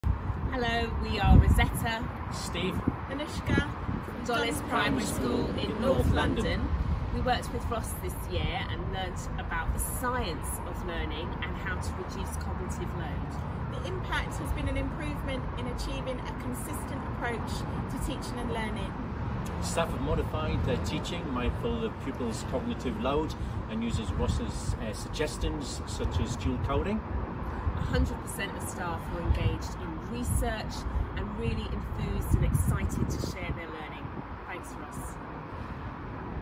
Guide To Memory, Teacher Training
In March, May and June 2023 I was invited to work with the teachers and governors at Dollis Primary, Mill Hill, London.

This short video is an endorsement from headteacher Rosetta Dyer (middle) and deputy headteachers, Steve Lee (left) and Anouska Sehmi (right)

Anouska said: "The impact has been achieving a consistent approach to teaching and learning."